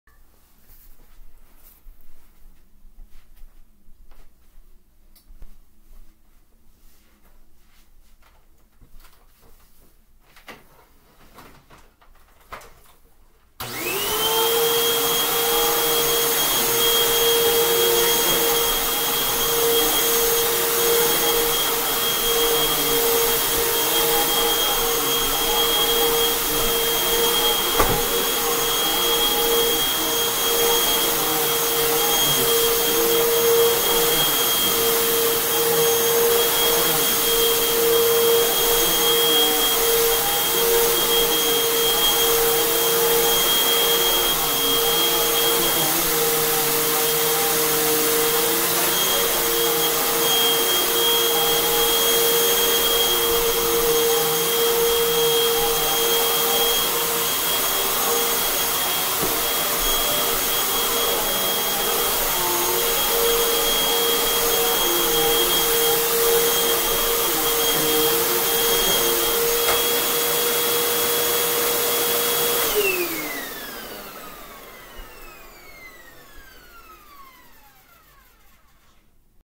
Christmas Eve Vacuuming with 2014 Hoover Windtunnel 2 Rewind (UH70825).
Shout out everybody.
Decided to make a Video for Christmas Eve using one of my Vacuums.
Have a Merry Christmas and Happy New Year everybody.